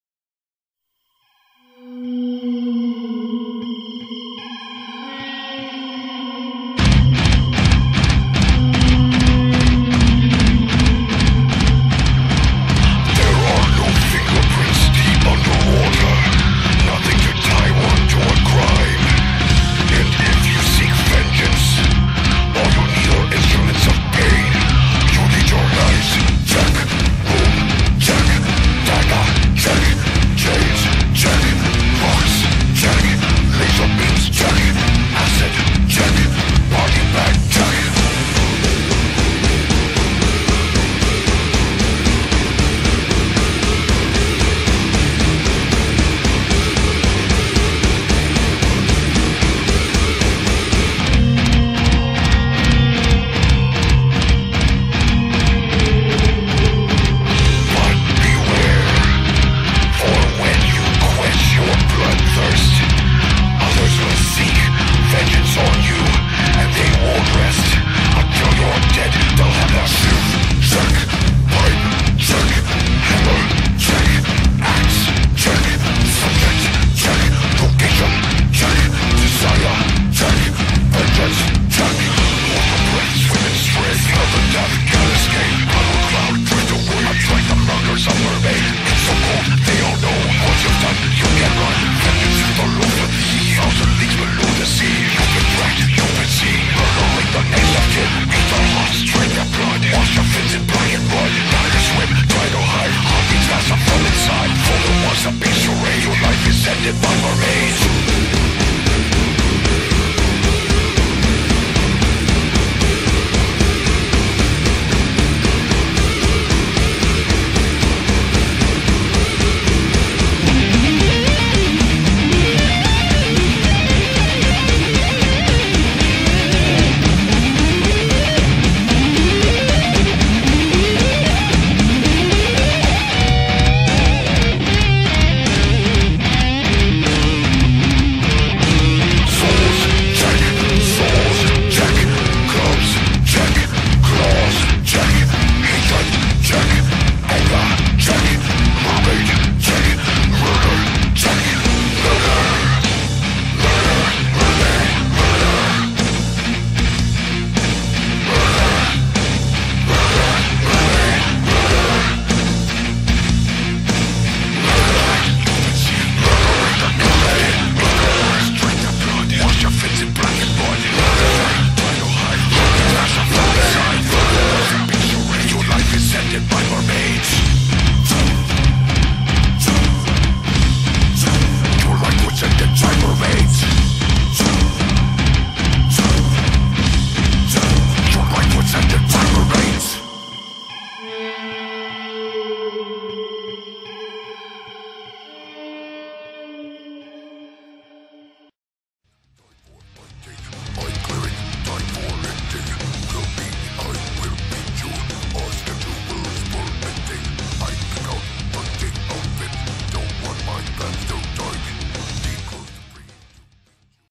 RED MEDUSA in Bat Metal.
Interpretação dos estúdios Red Medusa para um concerto muito Louco de Batman , Robin, e os personagens de Bob eSponja.